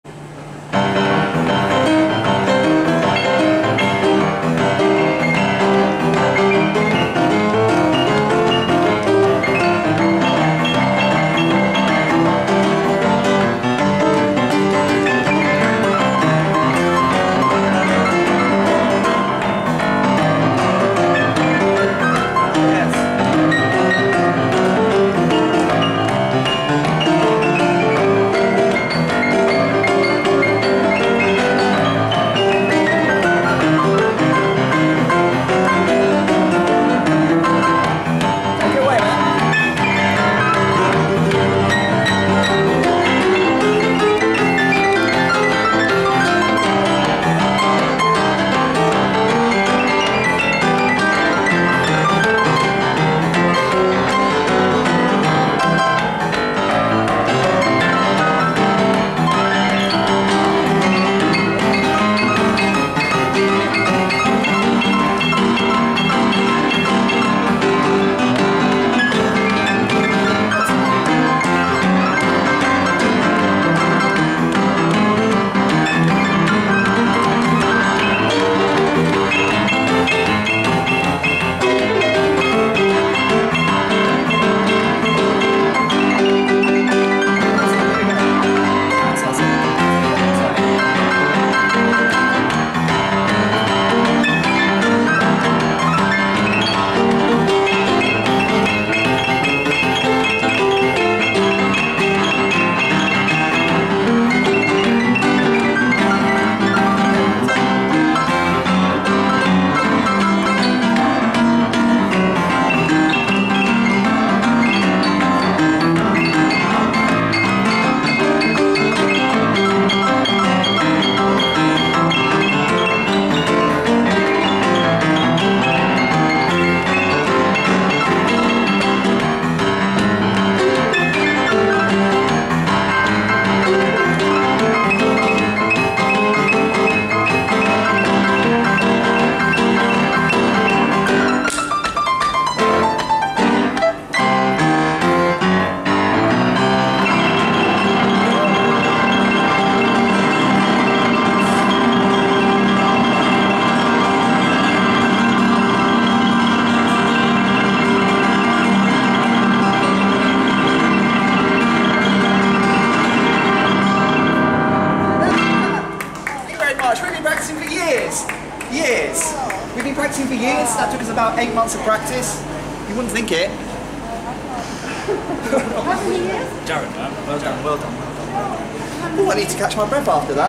Fan Plays An Amazing Boogie Blues Duet
Random boogie woogie duet with a fan at the station piano. Enjoy!